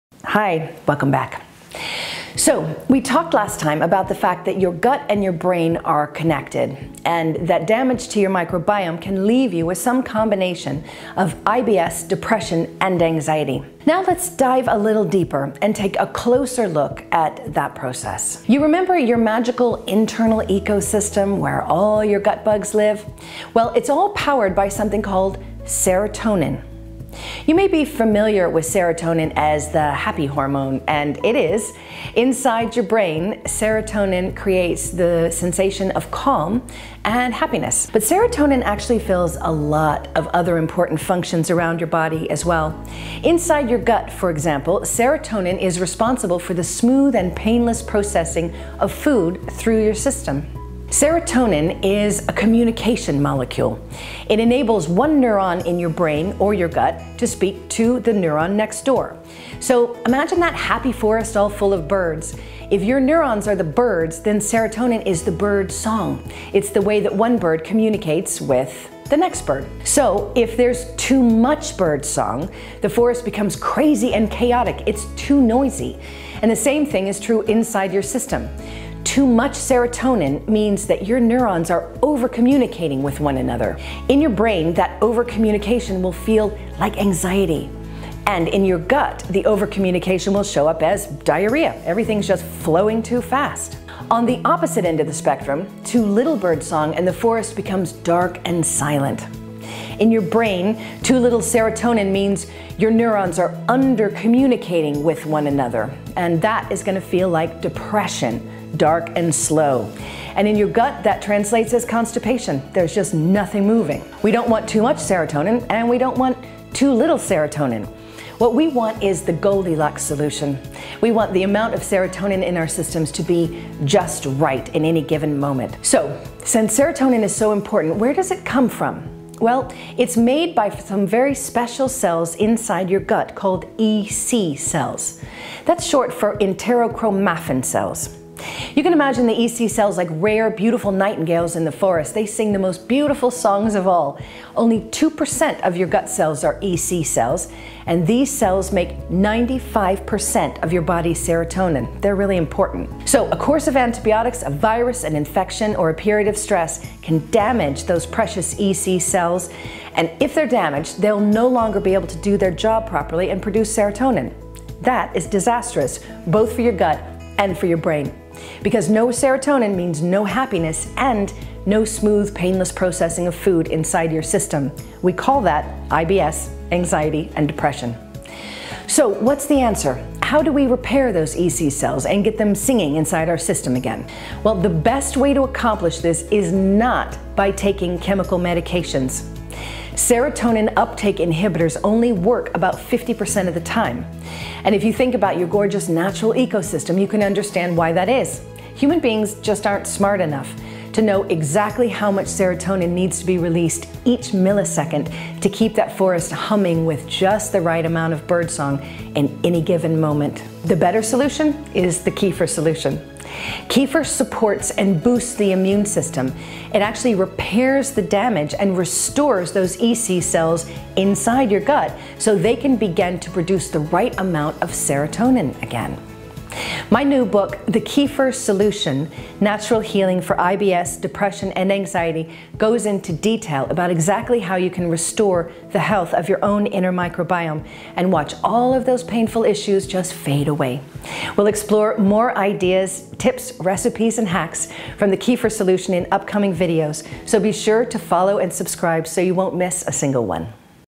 Ep 3 | What Is Serotonin?
Serotonin is the magic molecule that creates happiness inside your brain, as well as smooth, painless functioning inside your gut. Damage to this serotonin system can harm your mental health, resulting in IBS, depression and anxiety.